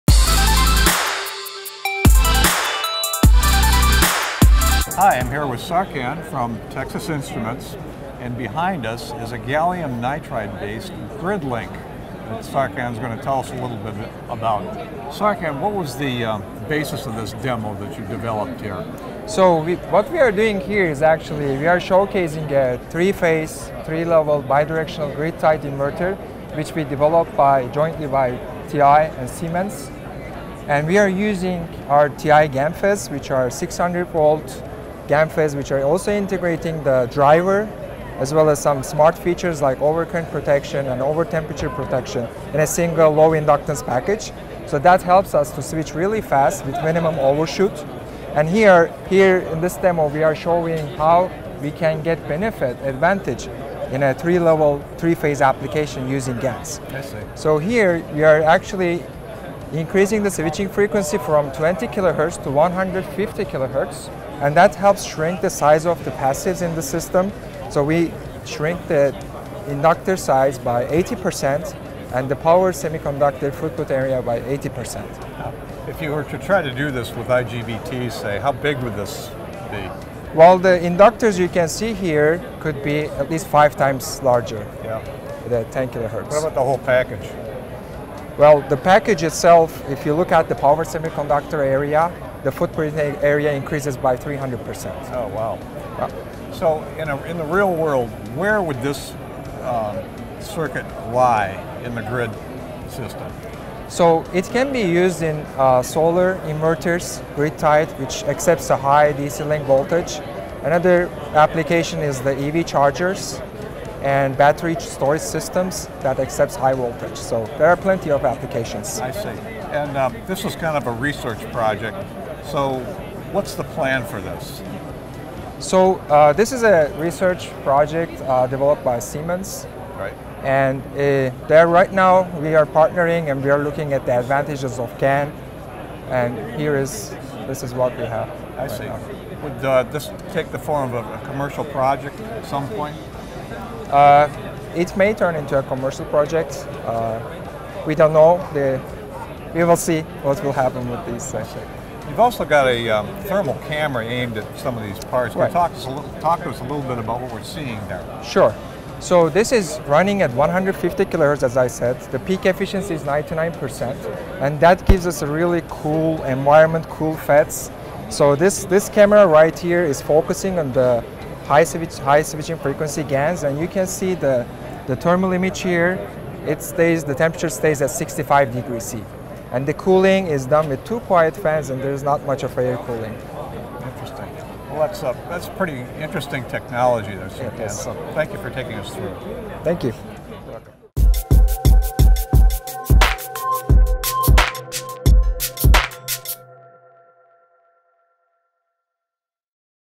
Grid links get more compact thanks to gallium nitride FETs
A grid link inverter can send current back and forth between the electrical grid and facilities that can both use power and generate it, as with buildings containing solar arrays. TI's Serkan Dusmez explains a new design for a grid link circuit, developed with Siemens, that uses 600-V GaN FETs switching at 150 kHz to reduce the size of passive components by up to 80%.  The GaN FETs on the three-phase, three-level bidirectional circuit are packaged with their own gate drivers to minimize parasitic elements and the resulting overshoot voltages.